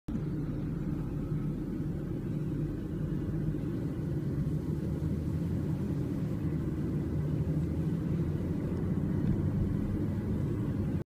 Ekologické vozidlo v Praze
I tahle auta jezdí ještě po silnici. Natočeno v Praze. Za autem se díky dýmu nedalo ani jet, tak jsem vám ho nahrál:) - Autor: Pavel Pefek